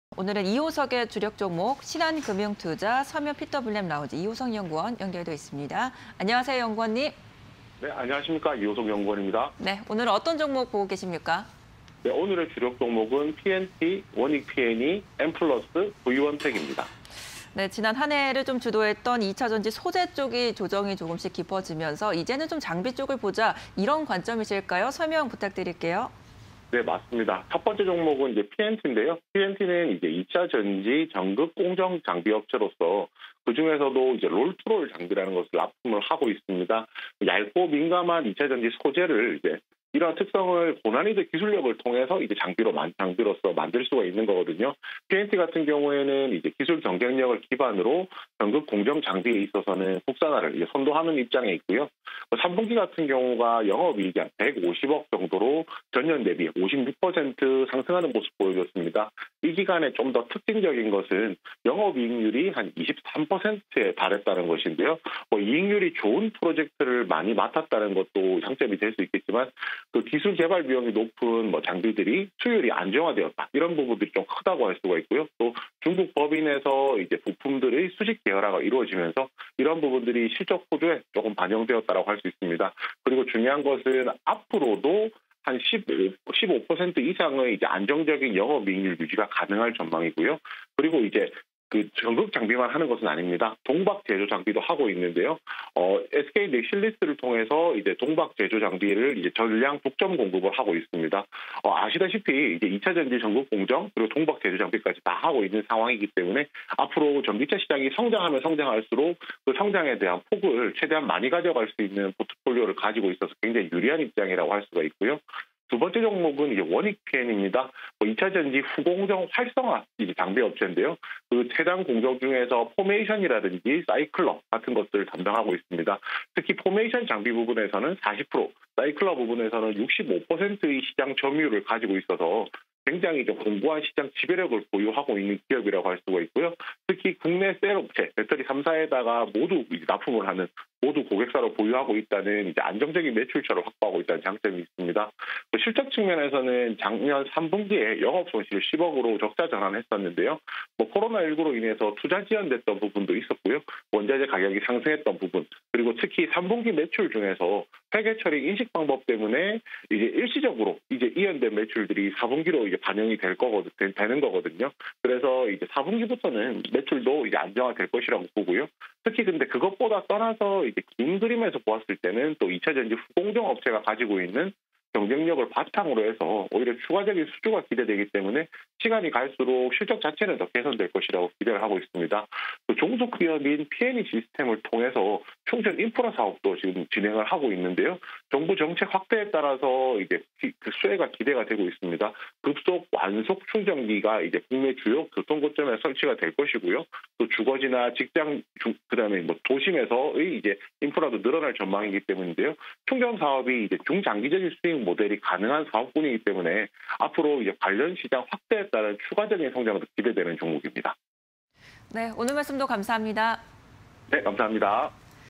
피엔티, 원익피앤이, 엠플러스, 브이원텍_이호석의 주력 종목 (20220111)
1. 피엔티(137400)
2. 엠플러스(259630)
3. 원익피앤이(131390)
4. 브이원텍(251630)

피엔티(137400)
- 2차전지 전극 공정 장비 업체
- 장비 업종 內 독보적 영업이익률… 23.6%
- 동박제조 장비 독점 공급 등 포트폴리오 다변화

원익피앤이(131390)
- 2차전지 후공정 활성화 장비 업체
- 후공정 경쟁력 바탕 추가 수주 확대 기대
- 종속기업 피앤이시스템즈